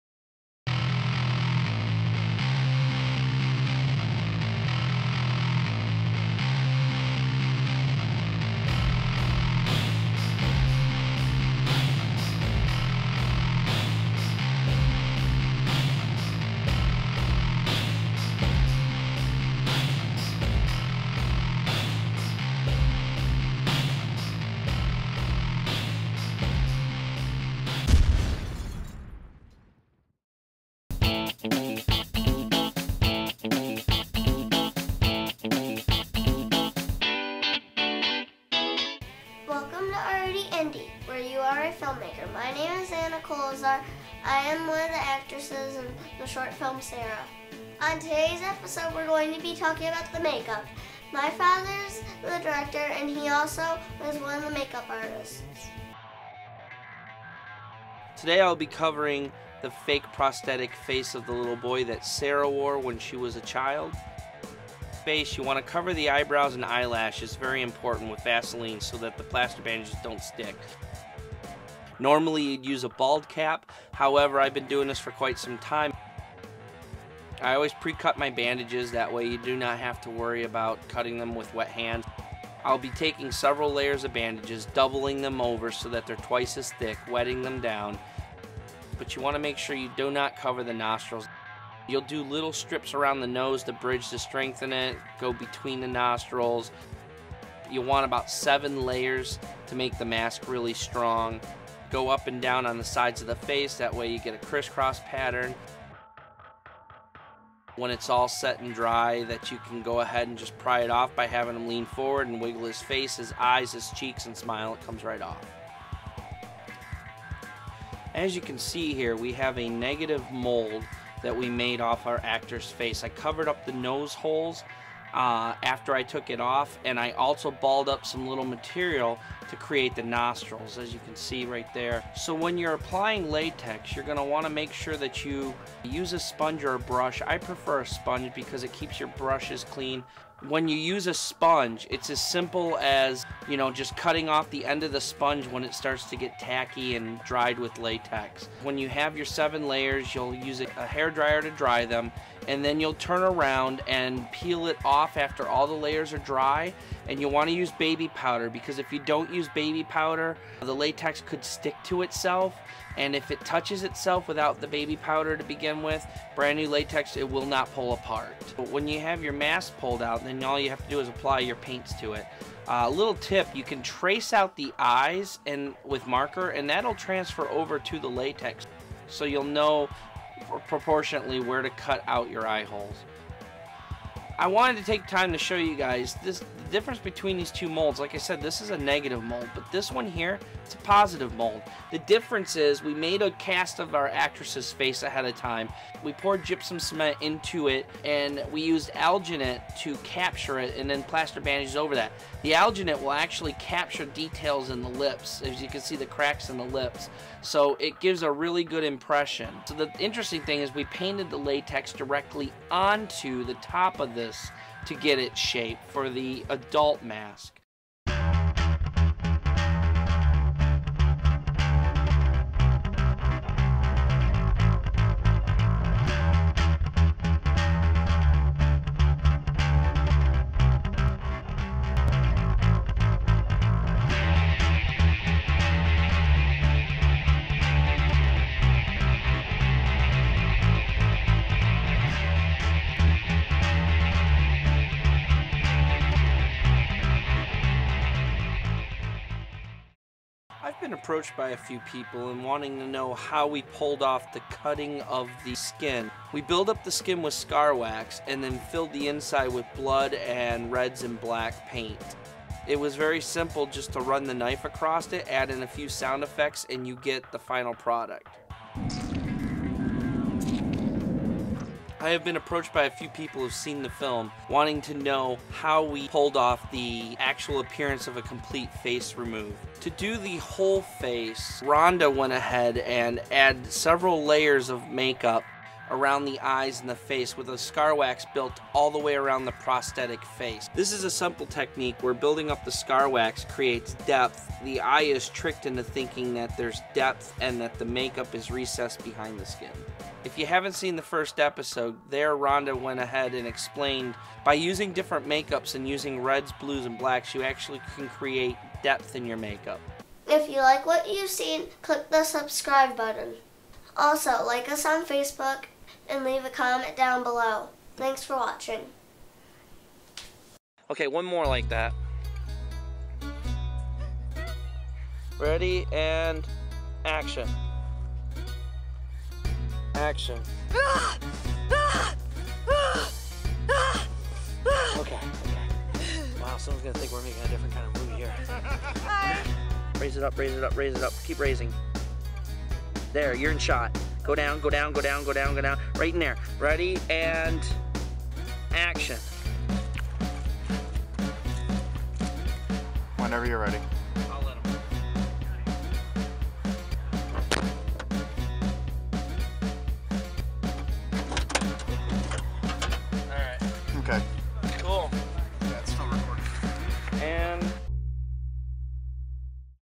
Face Off and Casting
This episode of Already Indie is the 4th and final episode covering the short film Sara. We talk about plaster casting and behind the makeup effects of a character without a face.